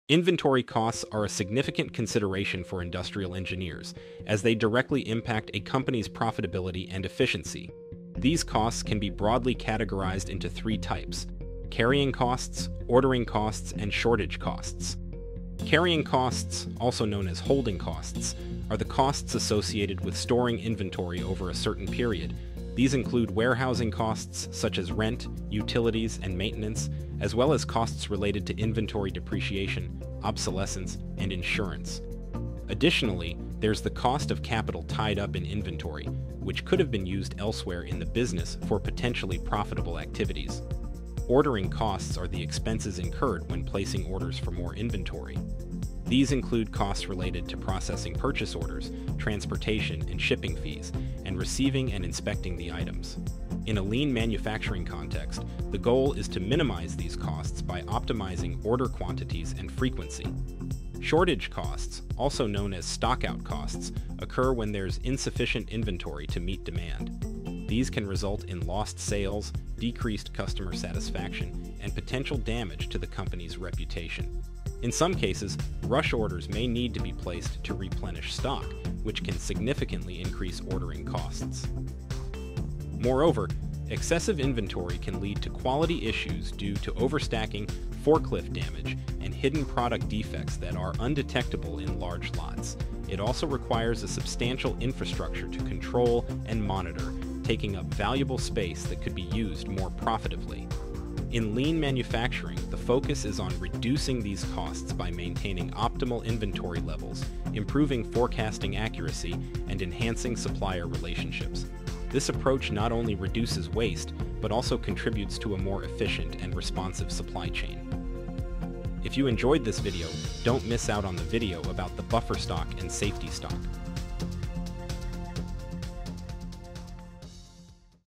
Inventory Costs Explained | Carrying, Ordering and Shortage Costs
Inventory costs are critical for industrial engineers, impacting profitability. They include carrying, ordering, and shortage costs.

Carrying costs involve storage expenses and capital tied up in inventory.

Ordering costs arise from placing orders, while shortage costs result from insufficient inventory.

Lean manufacturing aims to minimize these costs through optimal inventory levels and improved processes.

Inventory costs are a significant consideration for industrial engineers, as they directly impact a company's profitability and efficiency.

These costs can be broadly categorized into three types: carrying costs, ordering costs, and shortage costs.

▶ Carrying Costs

Carrying costs, also known as holding costs, are the costs associated with storing inventory over a certain period.

These include warehousing costs such as rent, utilities, and maintenance, as well as costs related to inventory depreciation, obsolescence, and insurance.

Additionally, there's the cost of capital tied up in inventory, which could have been used elsewhere in the business for potentially profitable activities.

▶ Ordering Costs

Ordering costs are the expenses incurred when placing orders for more inventory.

These include costs related to processing purchase orders, transportation and shipping fees, and receiving and inspecting the items.

In a lean manufacturing context, the goal is to minimize these costs by optimizing order quantities and frequency.

▶ Shortage Costs

Shortage costs, also known as stockout costs, occur when there's insufficient inventory to meet demand.

These can result in lost sales, decreased customer satisfaction, and potential damage to the company's reputation.

In some cases, rush orders may need to be placed to replenish stock, which can significantly increase ordering costs.

Moreover, excessive inventory can lead to quality issues due to overstacking, forklift damage, and hidden product defects that are undetectable in large lots.

It also requires a substantial infrastructure to control and monitor, taking up valuable space that could be used more profitively.

▶ Conclusions

In lean manufacturing, the focus is on reducing these costs by maintaining optimal inventory levels, improving forecasting accuracy, and enhancing supplier relationships.

This approach not only reduces waste but also contributes to a more efficient and responsive supply chain.

Hi,